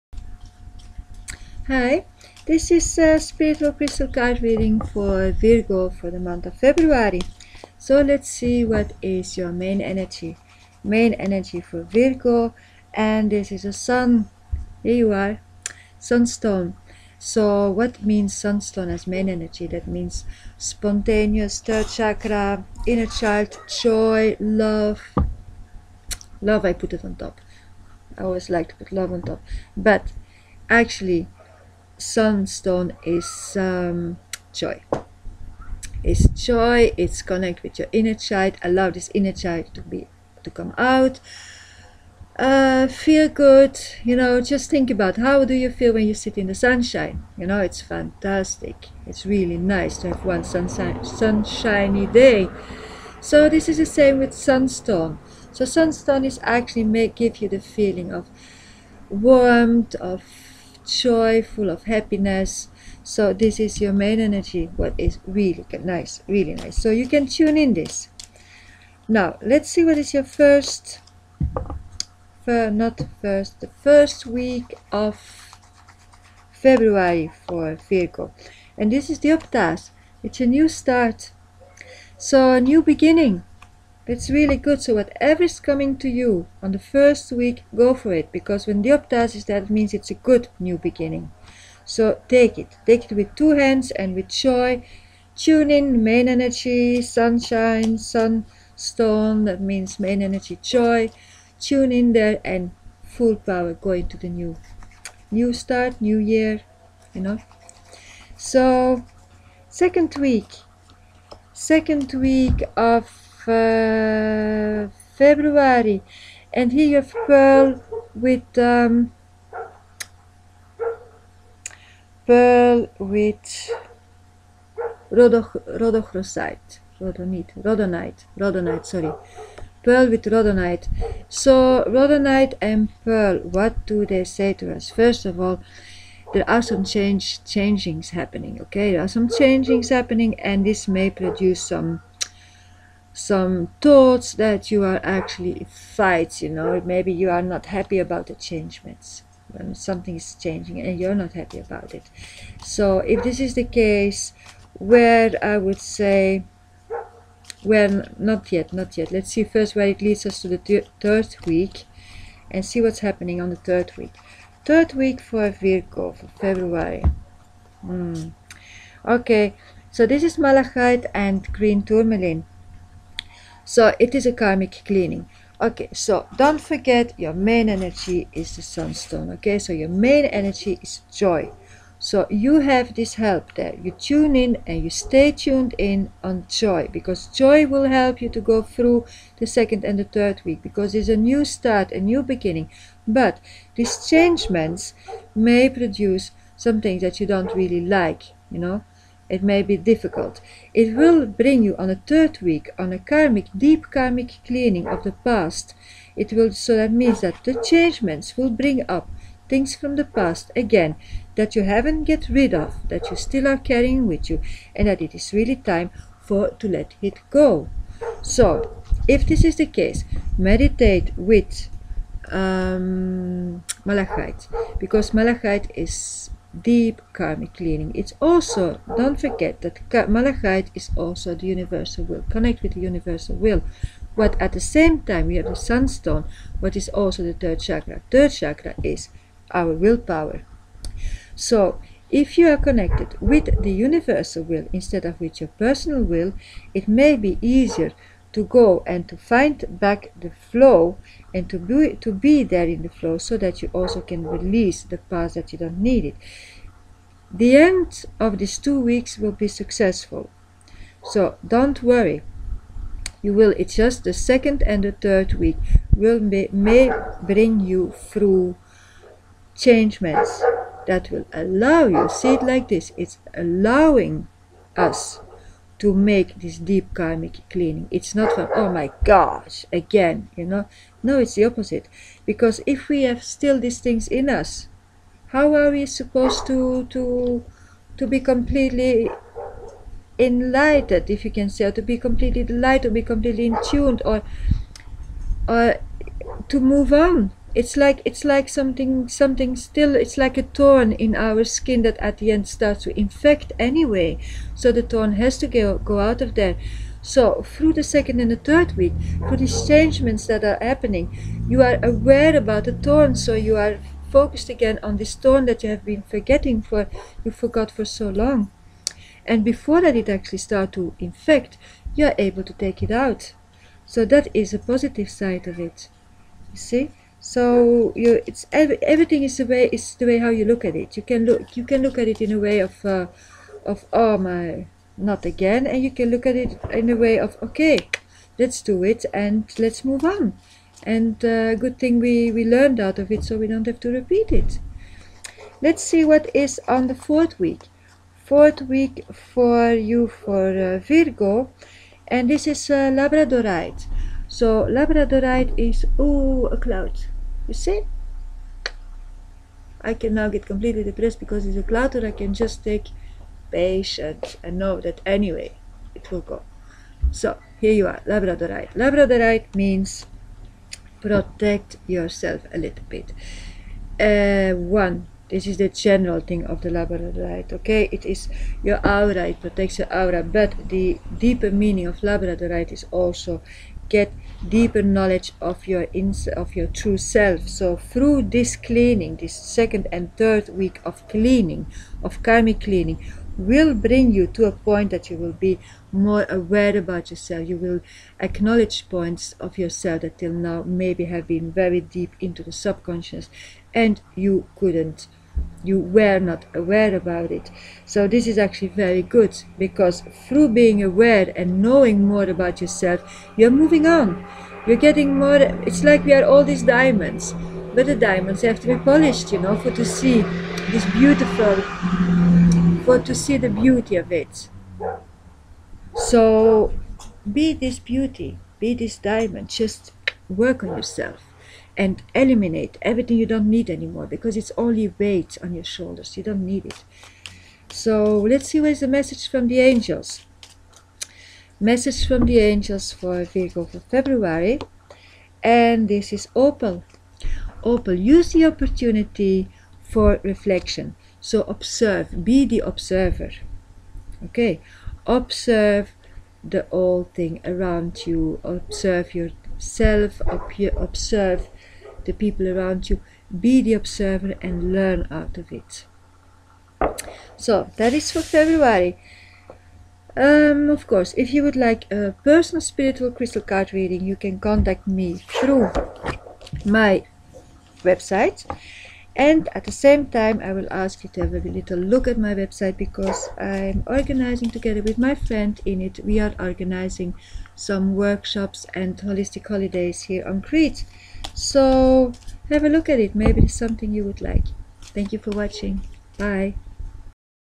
Virgo, February; Spiritual Crystal Card Reading Horoscope 2015
This is a Spiritual Crystal Card reading that will guide you on your inner path of truth.